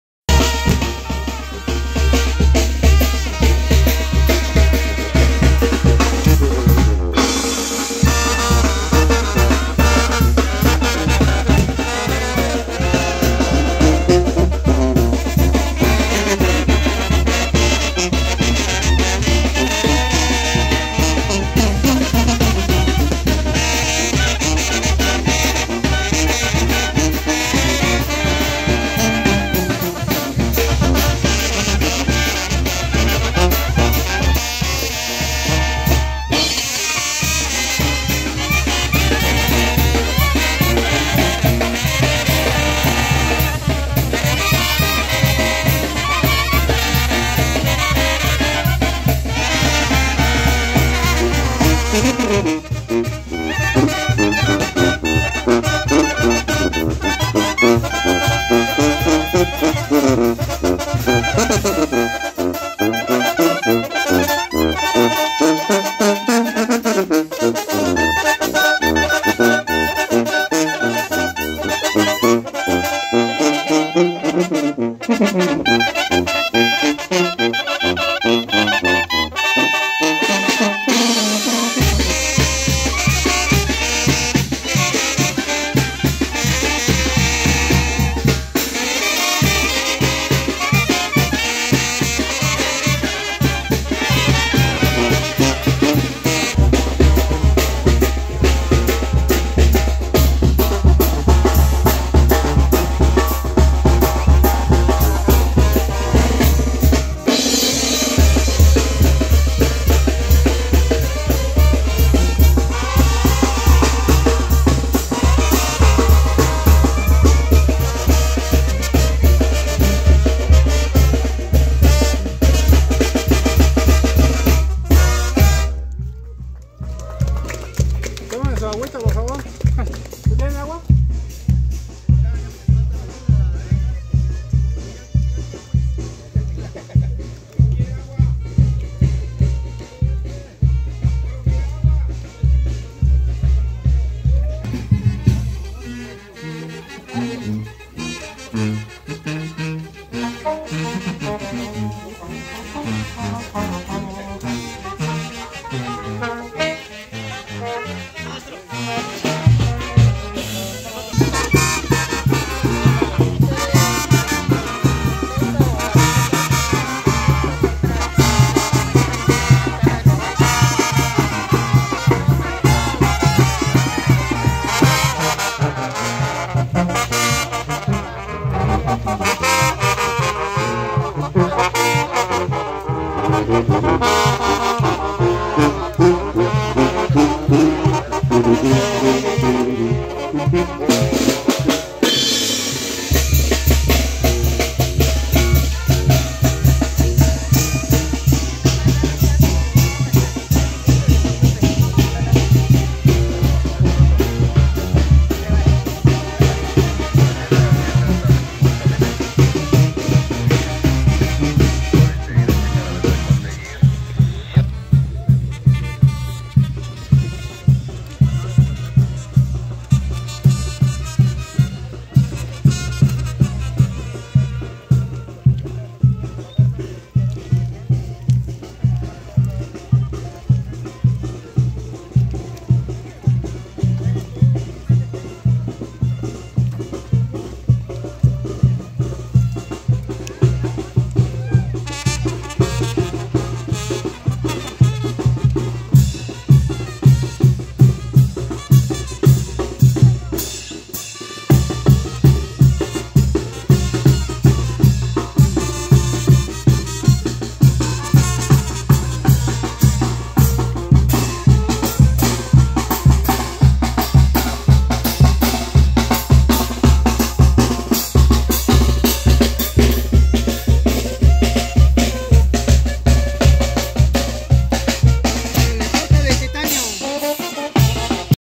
PEREGRINACIÓN EN CALENDA A EL CHAMIZAL, TICUÁ 2023
Fiesta en honor a la Virgen de Santa Cecilia en el Chamizal, Santa Catarina Ticuá